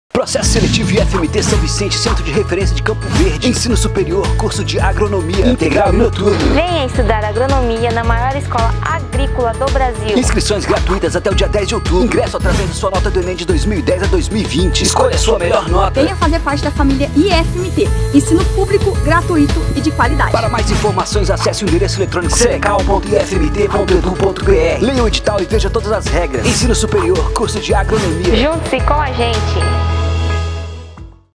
Processo Seletivo 2022/1 - Bacharelado em Agronomia
BACHARELADO EM AGRONOMIA

IFMT tem 1.189 VAGAS abertas para cursos superiores

No IFMT São Vicente são 50 vagas para o curso BACHARELADO EM AGRONOMIA, ministrado no Centro de Referência de Campo Verde.

25 delas são para o período INTEGRAL e as outras 25 são para o NOTURNO.

As inscrições são GRATUITAS e estarão abertas no período de 02 de setembro a 10 de outubro, exclusivamente pelo

Seleção
Para participar deste processo seletivo é necessário ter participado de, pelo menos, uma das edições do Exame Nacional do Ensino Médio (Enem). O candidato deverá se inscrever identificando o seu número de inscrição do Enem (2010 a 2020).

A seleção será realizada mediante a nota obtida no Enem. O resultado da classificação será feito pelo somatório da nota de avaliação objetiva com a nota da avaliação discursiva (redação).